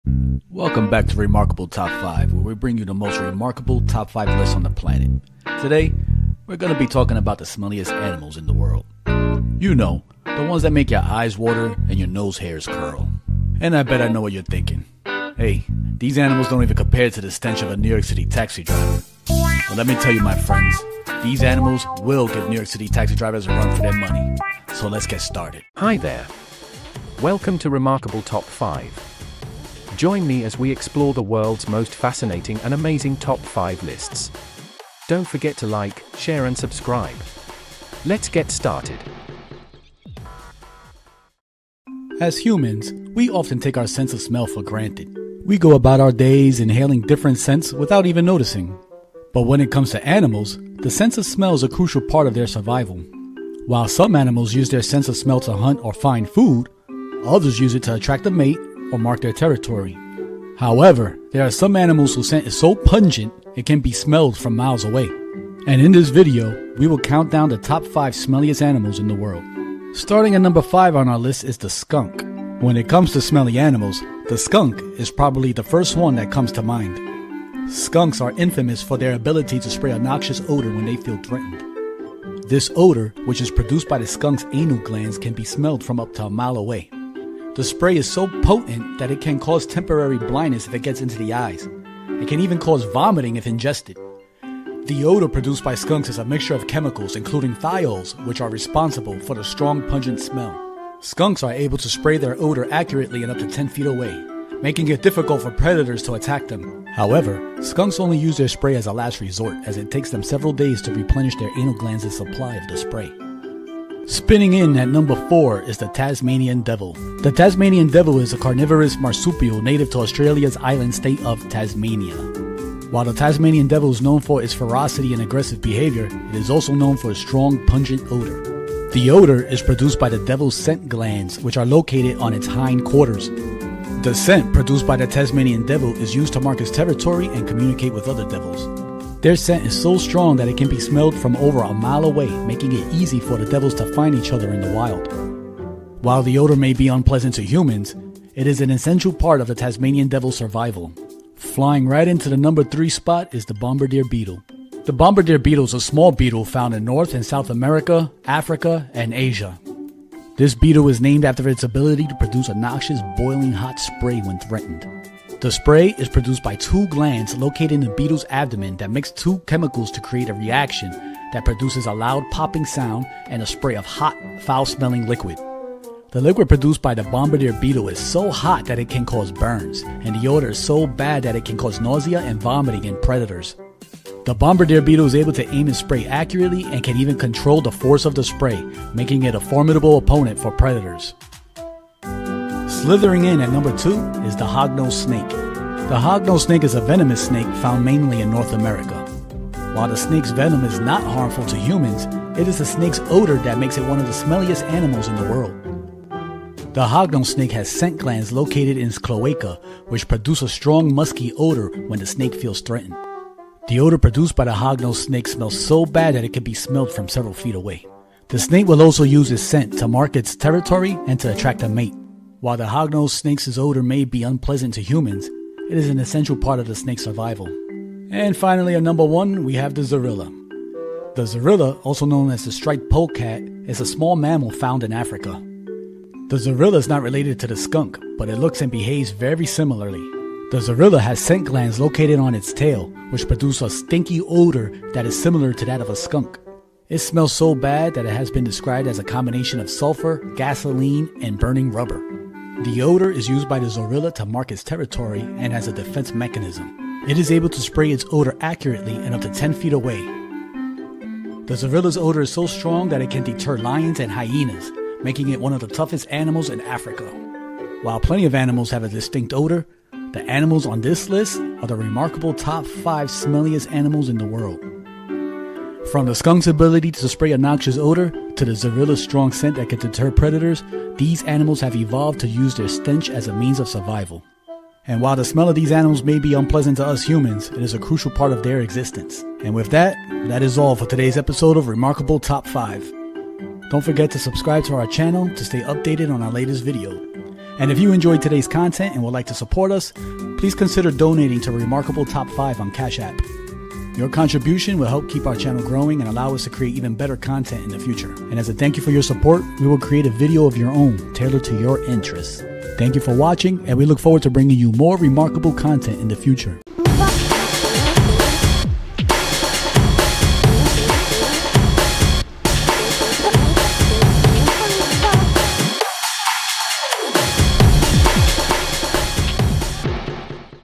Remarkable Top 5 Smelliest Animals On The Planet
Welcome to Remarkable Top 5! In this episode, we're taking a closer look at the top 5 smelliest animals in the world. From the foul odor of a skunk to the pungent scent of a zorilla, these animals are known for their distinctive and sometimes overwhelming smells.